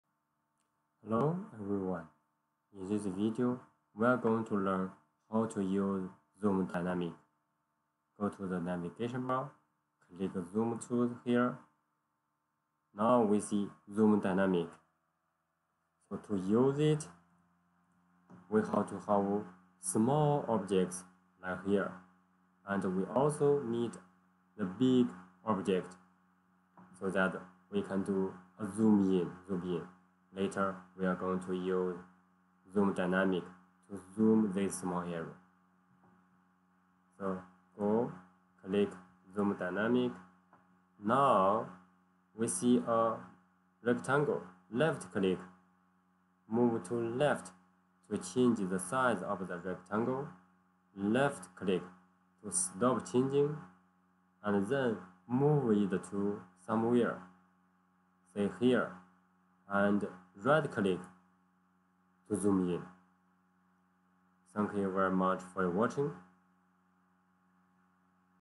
AutoCAD 2021 Tutorial: Zoom tool (VI): how to use Zoom Dynamic
Go find the Zoom Dynamic tool in the Navigation bar on your right